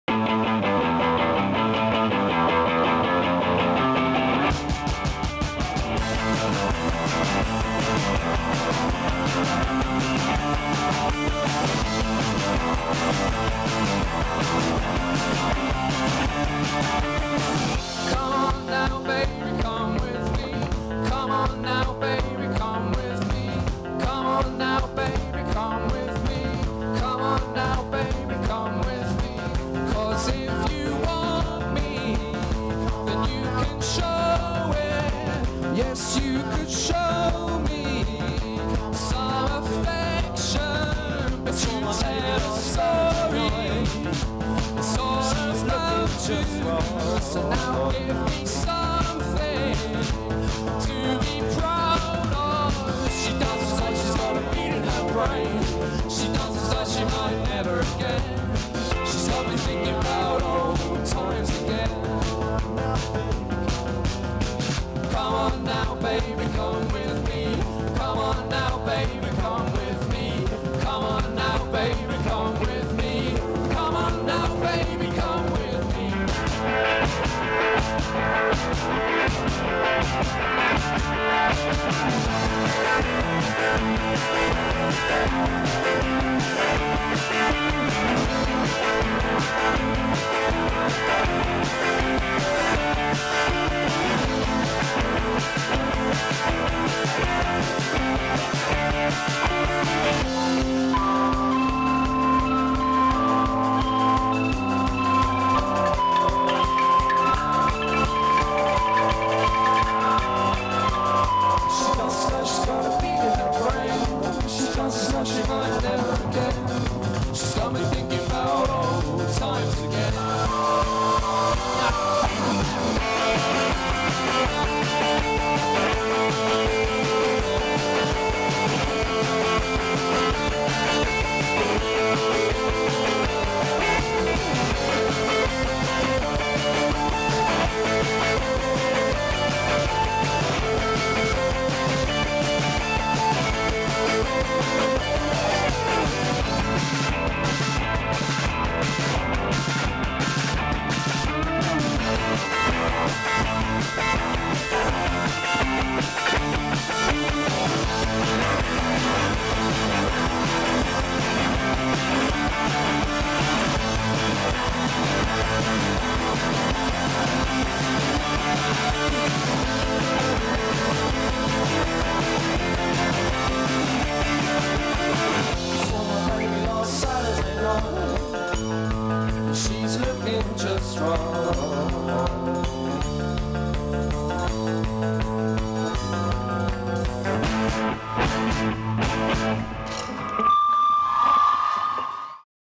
Pete and the pirates - Fnac Indétendances 2008 (2/2)
Le Festival Fnac Indétendances 2008, ce sont 9 soirées de concerts et plus de 35 artistes. Entre le 25 juillet et le 16 août 2008, assistez gratuitement aux concerts au Pont de Sully (Metro Sully-Morland).